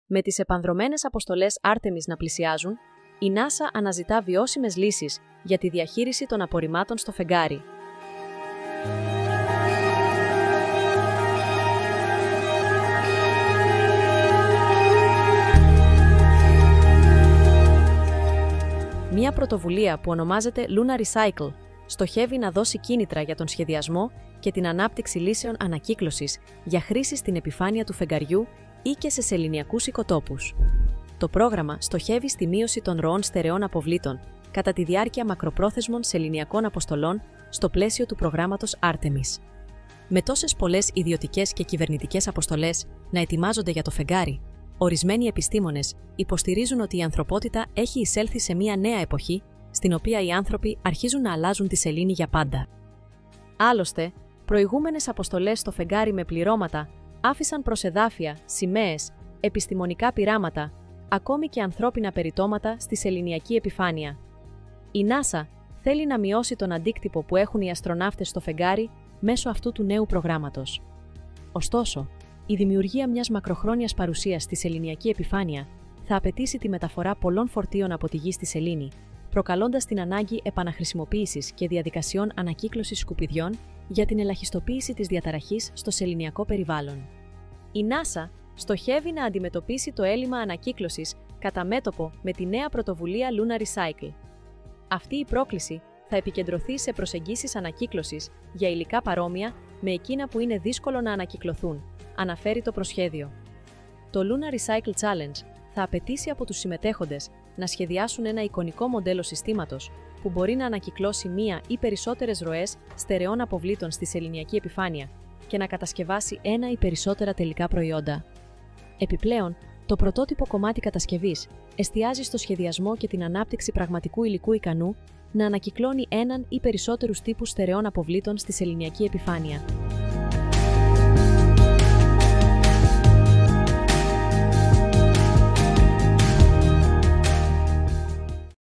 Η NASA εξετάζει την ανακύκλωση σκουπιδιών στο φεγγάρι
Η NASA εξετάζει την ανακύκλωση σκουπιδιών στο φεγγάρι. Με τις επανδρωμένες αποστολές Artemis να πλησιάζουν, η NASA αναζητά βιώσιμες λύσεις για τη διαχείριση των απορριμμάτων κατά τη διάρκεια των μακροπρόθεσμων αποστολών στο φεγγάρι, όπως την ανακύκλωση των σκουπιδιών.

00:00 Εισαγωγή
00:31 Στόχος προγράμματος
01:11 Ανάγκη ανακύκλωσης
01:44 LunaRecycle Challenge